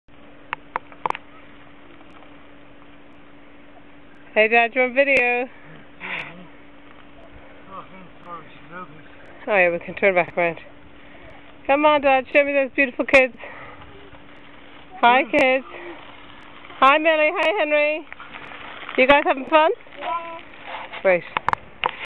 Cycling in Gundy
Gundy is at the base of the stunning barrington tops area